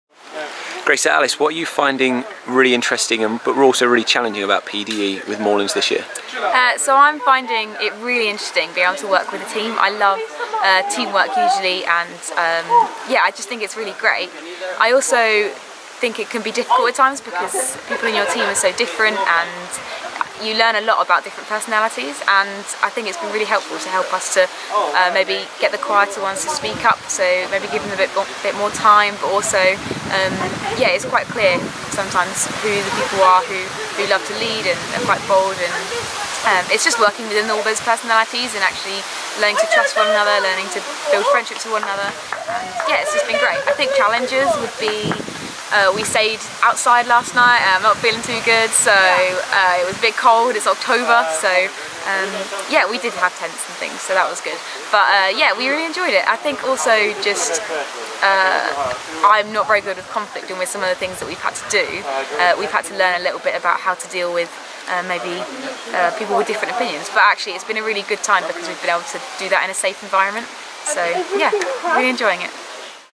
The adventure of PDE 2016
Each year, our second year BA students embark on a Personal Development Exercise (PDE) at Avon Tyrell outdoor activity centre, Hampshire. Notoriously, the events in this annual exercise are kept ‘secret’ so future years go into it unknowing, but in this snippet with Alice Fenning, we get a sneak peek of what our students encounter in their three days away.